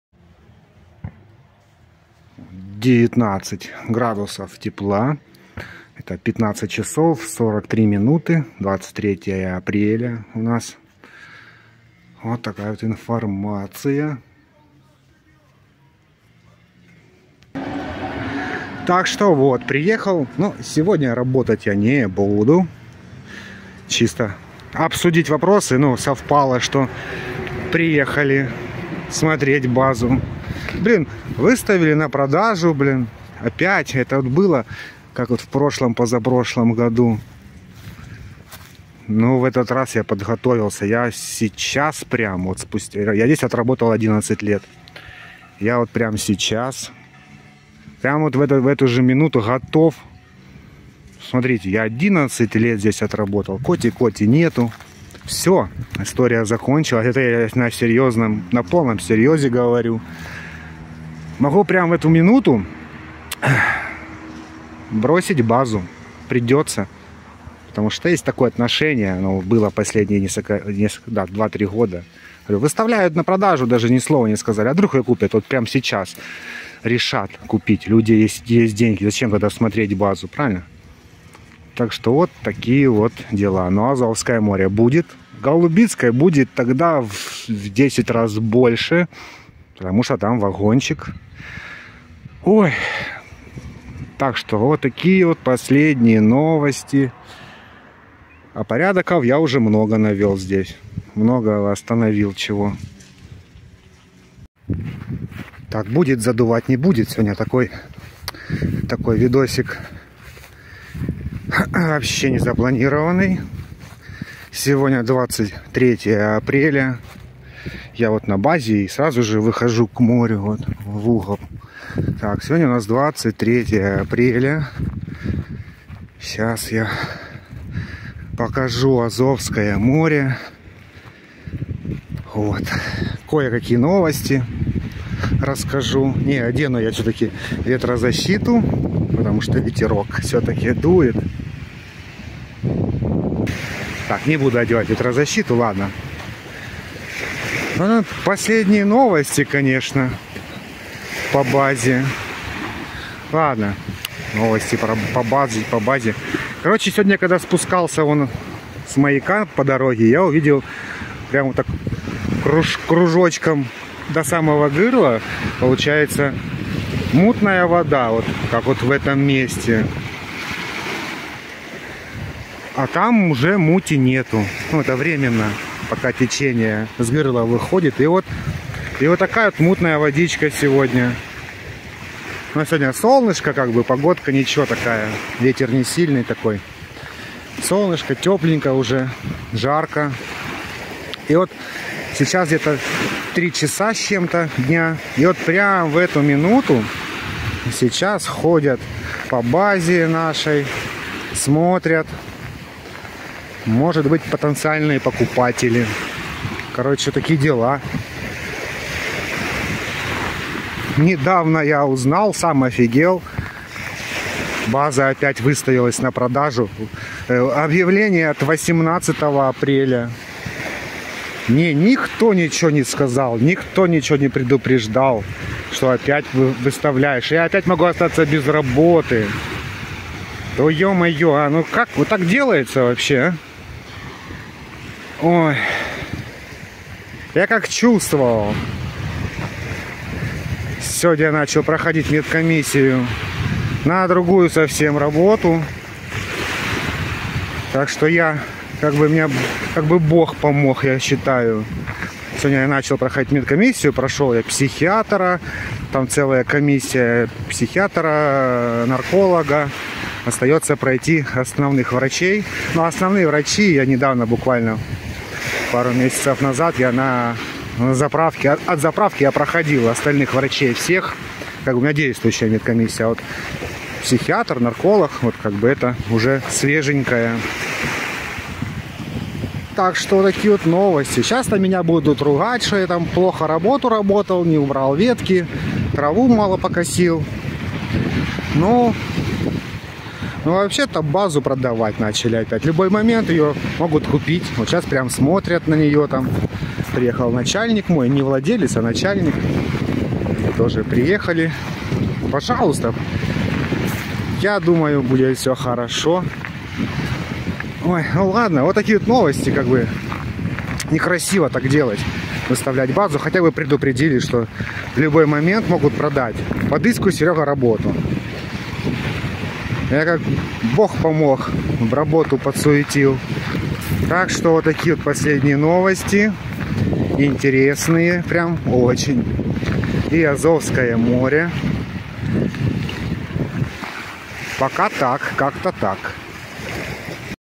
Море мутное, база отдыха продается, я ухожу!
Продается база отдыха в поселке Пересыпь. Азовское море, погода.